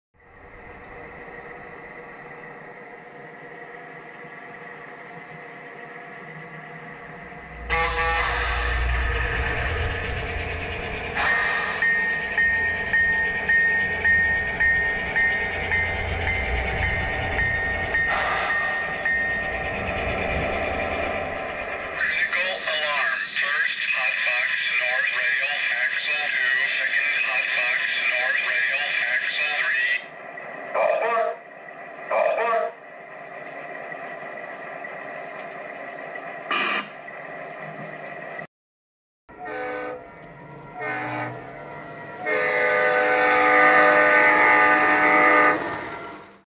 Phoenix Big Sound EMD E8
Phoenix BigSound P8

Quick video showing the different sounds from the Phoenix sound system. The sounds can be downloaded from the Phoenix website for the E8.